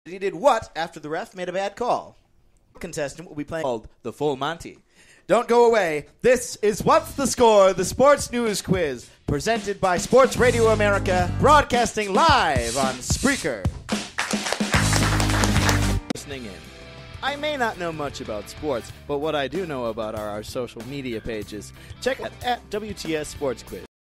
What's the Score? The Sports News Quiz #11
Our Panelists this week are Grant Coppersmith, Chris Gibbons and Michael Cyr. Find out who wins our battle of the NFC North!